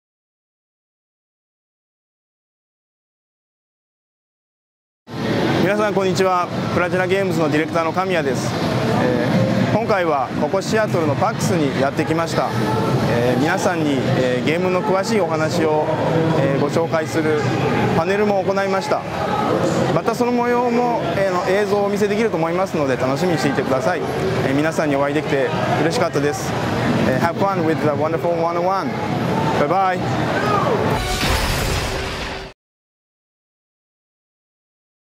Hideki Kamiya at PAX Prime 2013
Hideki Kamiya, creative director at PlatinumGames Inc. attended PAX Prime 2013 to interact with fans and talk about his new game The Wonderful 101.